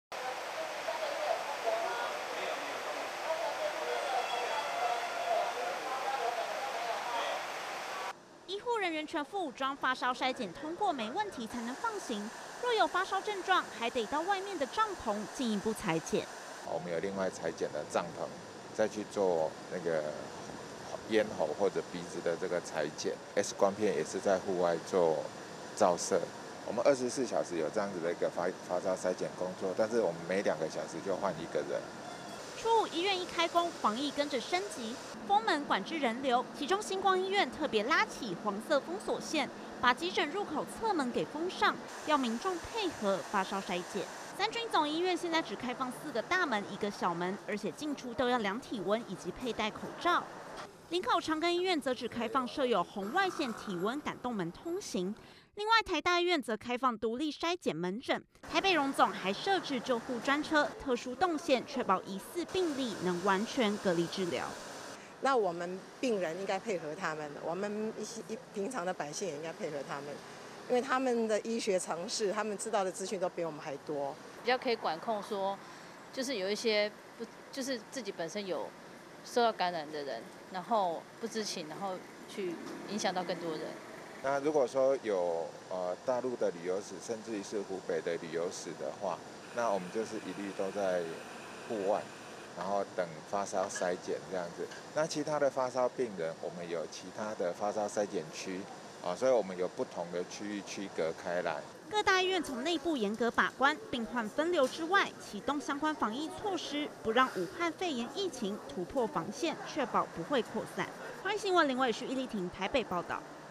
武漢肺炎疫情延燒 醫院封門 隔離專車接送｜寰宇新聞20200129
【寰宇新聞】

醫院初五開工後，為了防堵武漢肺炎，也祭出”封門”措施，像是林口長庚，封閉部分出入口，17道門只開5道門進出，以便集中管理人流，另外，新光醫院也在急診室外，架起帳棚，針對進出的民眾，嚴格要求戴口罩，通過發燒篩檢，而台北榮總則設有隔離救護專車，防疫總動員，就是要確保武漢肺炎，不擴散。